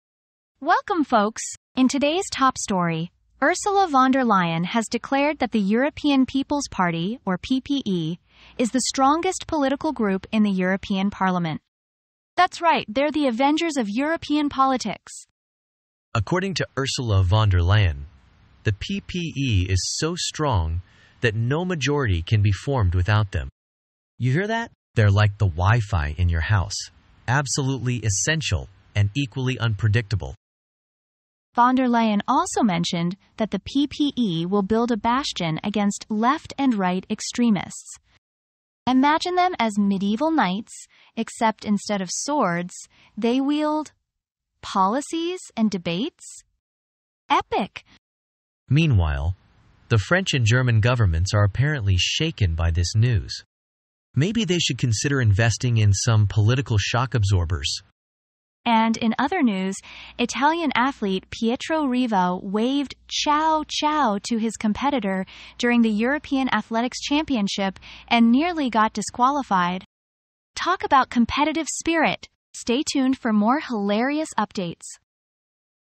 Von der Leyen Declares PPE: The Ultimate Political Party
Join us for a lighthearted and witty news report on Ursula von der Leyen's announcement about the PPE's dominance in the European Parliament.